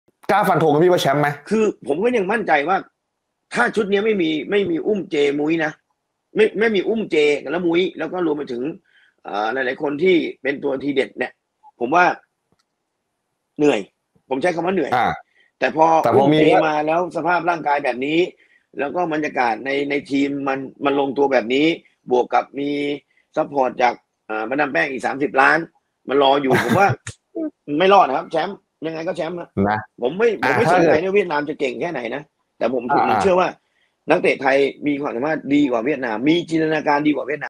highlight ตุงตาข่าย LIVE TALK
ฟัง โค้ชเตี้ย สะสม พบประเสริฐ ฟันธงว่าทีมชาติไทย จะคว้าแชมป์ AFF SUZUKI CUP 2020 ได้หรือไม่?